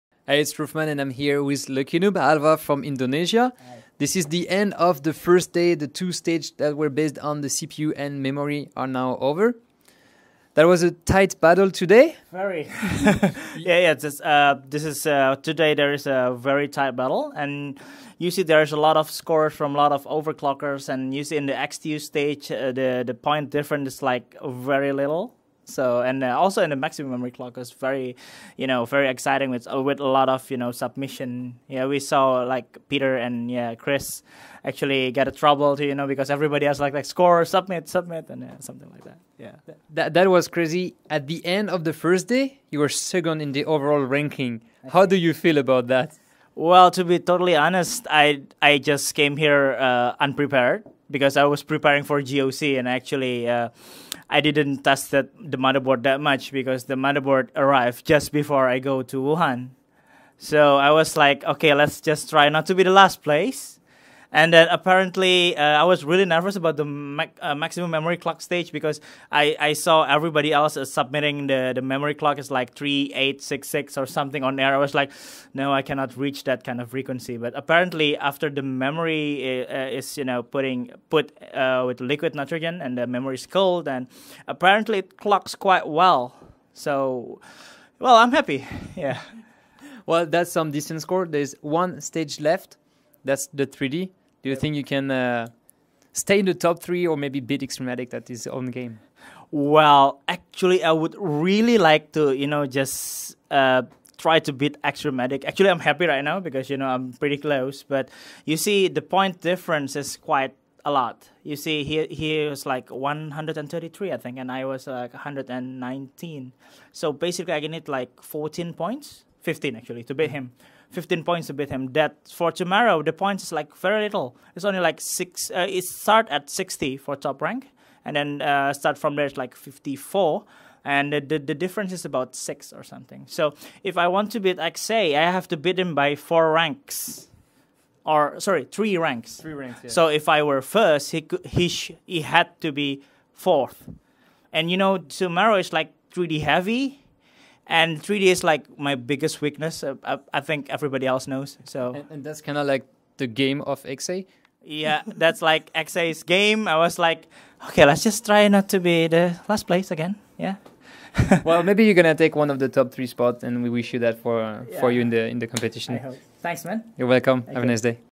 Lucky_noob - Third of Day 1 - H.O.T. Season III World Finals
Interview with Lucky_n00b (Alva Jonathan) from Indonesia, third after the first day of the HyperX H.O.T. Season III World Finals.

Video produced by OverClocking-TV.com.